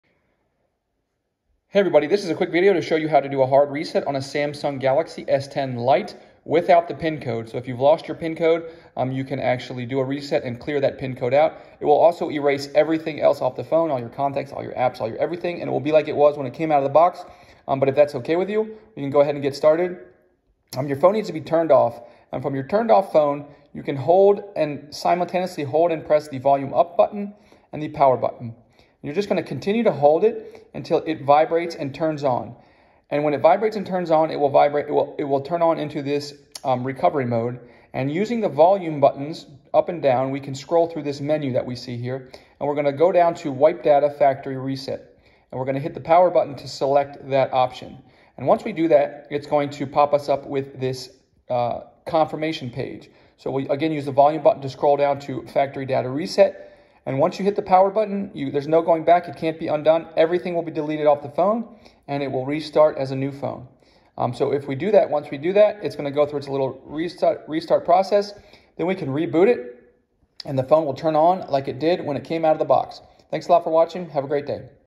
Hard Reset Samsung Galaxy S10 Lite Without Pincode
If you forgot your pin and the screen is locked you can hard reset your Samsung Galaxy S10 Lite mobile phone to its factory settings which will remove the pin and restore default settings on your phone.